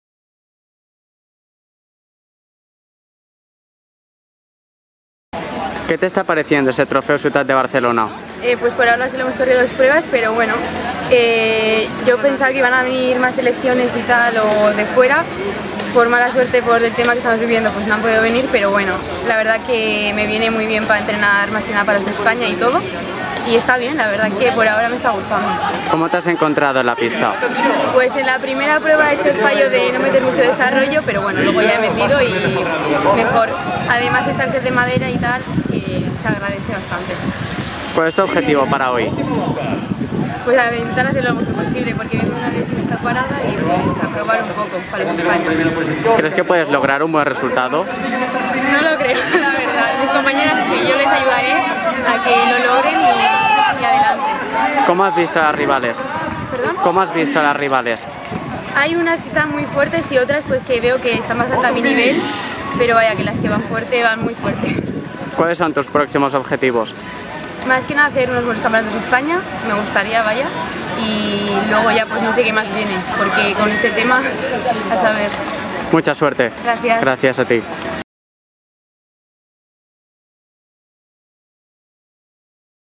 Entrevista a Garazi Estebez
Entrevista a Garazi Estebez, del Gipuzkoa Ogi Berri, en el Trofeu Ciutat de Barcelona de Pista, disputado en el Velòdrom d'Horta (Barcelona) el 12 de septiembre de 2020.